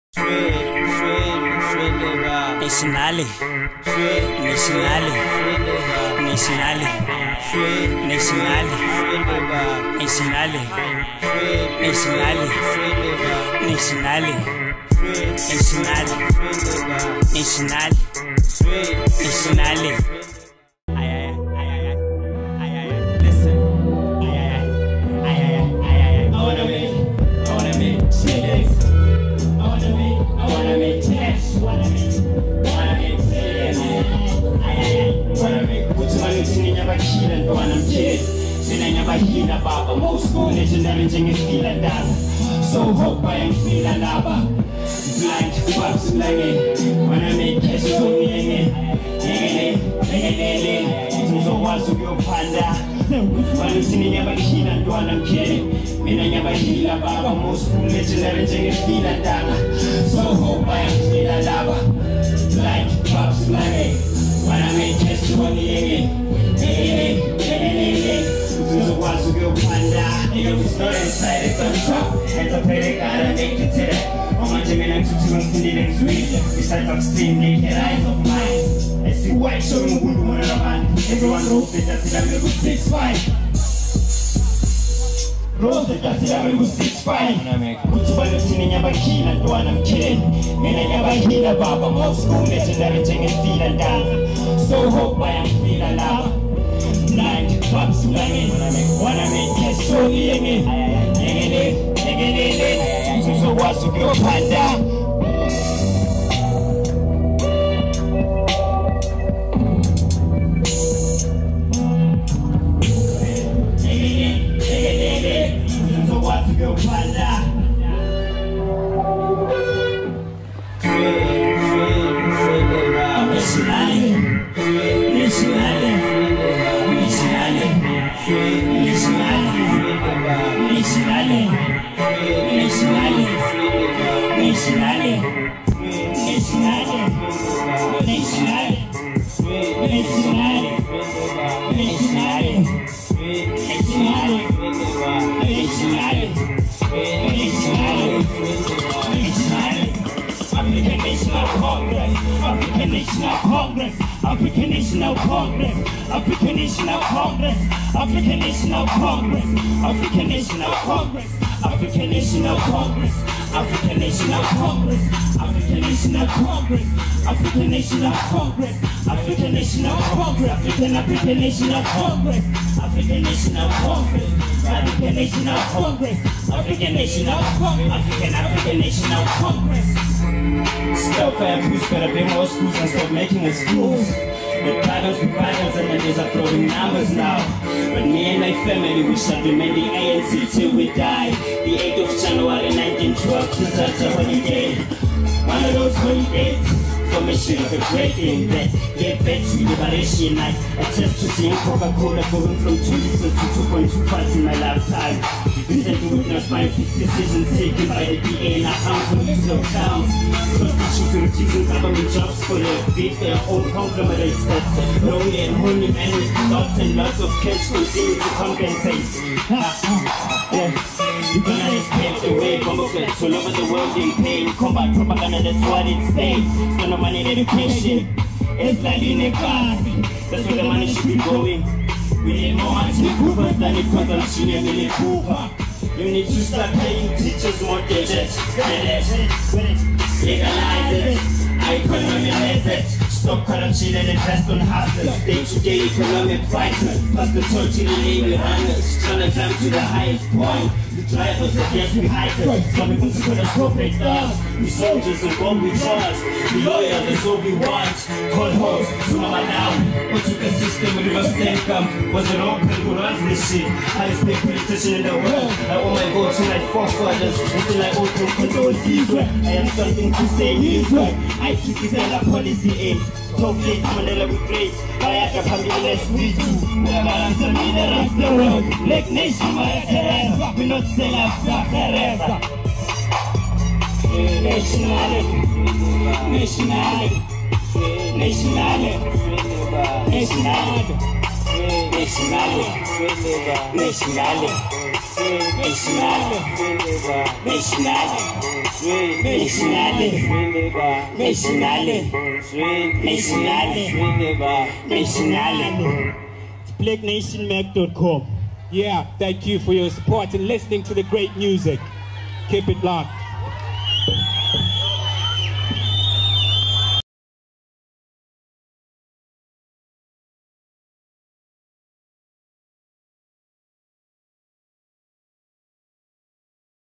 blacknationtv presents: Mankind performing at the Standerton Music Festival
Standerton Music Festival aims to bring the targeted community together and promote local talent.

As part of paying tribute or homage, Mankind made an amazing appearance at the festival...